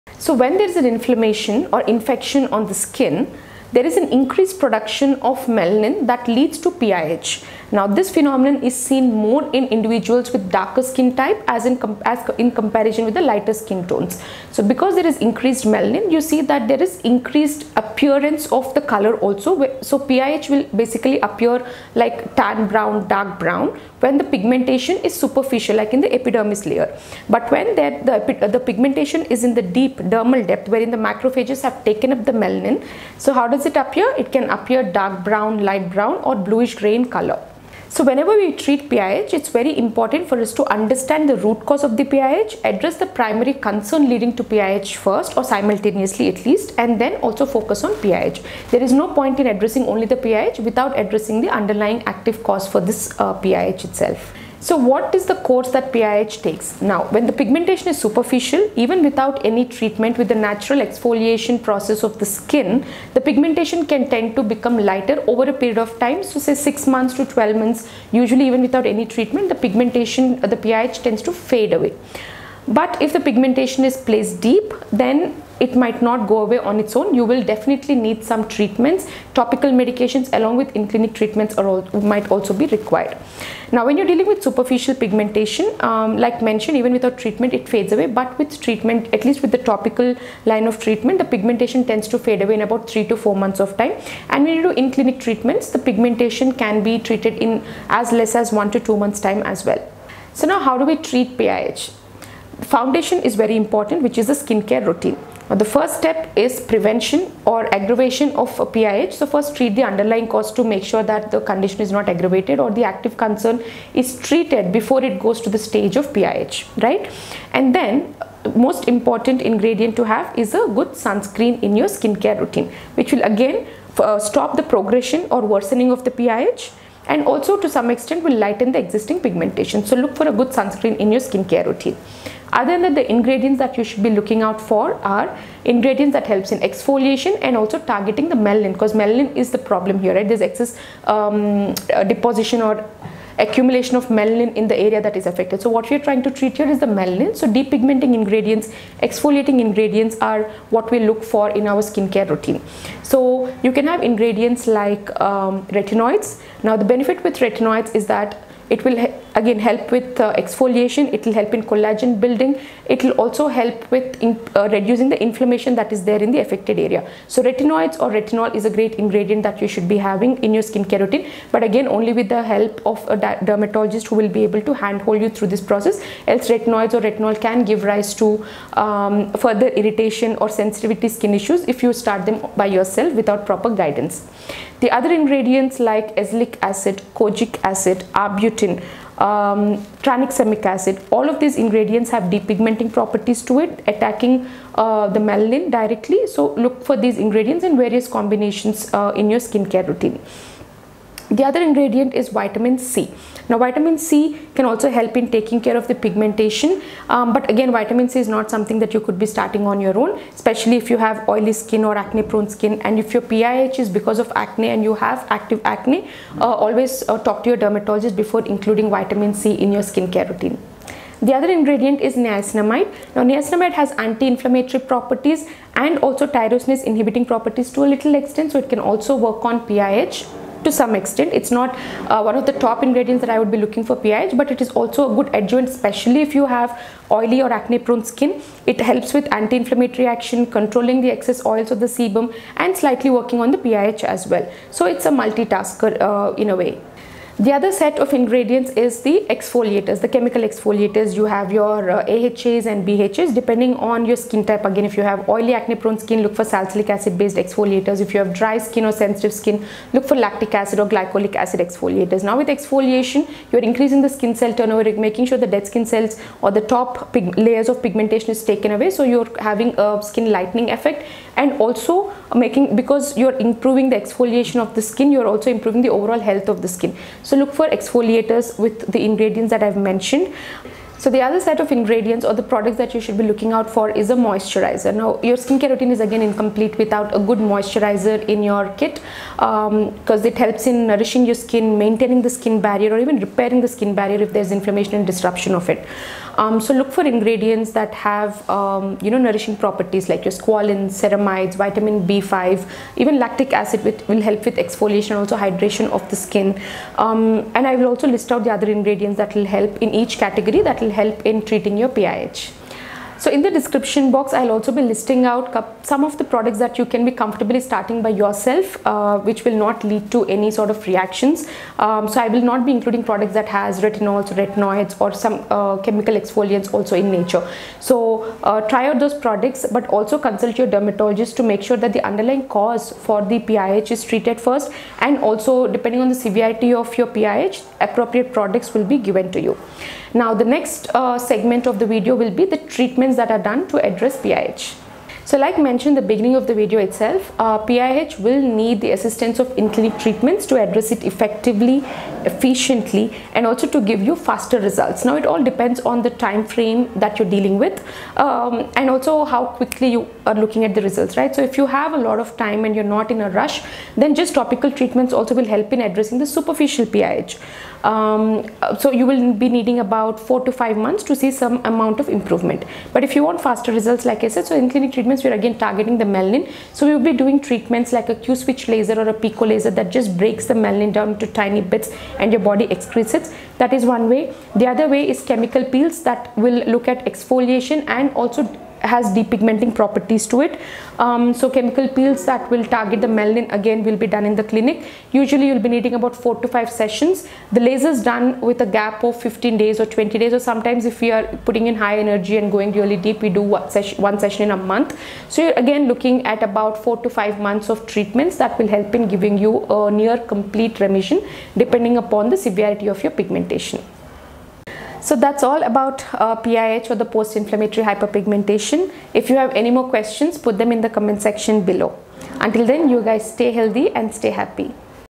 How to Treat Acne Scars? | Best Solution to Post Inflammatory Hyperpigmentation | Dr. Priyanka Reddy
Acne scars are formed when a breakout penetrates the skin deeply and damages the tissues beneath it. Before one tries to treat scars, it’s important to know what are its types. One such type of acne scars is Post-inflammatory Hyperpigmentation (PIH).

Post-inflammatory Hyperpigmentation (PIH) happens when the skin makes extra melanin after it has been irritated or injured. Melanin is a natural pigment that is responsible for the colour of the hair, skin, and eyes. PIH can affect either the epidermis — the skin’s surface level, or even the dermis — a deep layer of the skin.

PIH  can be prevented by following a good skincare routine & by including some of the vital ingredients. Watch the video to know more about PIH, its causes & how it can be prevented.

00:00 Introduction
00:06 What is PIH?
00:55 What is the course that PIH takes?
01:40 How do we treat PIH?
02:00 Most important ingredients
05:42 Treatments that are done to address PIH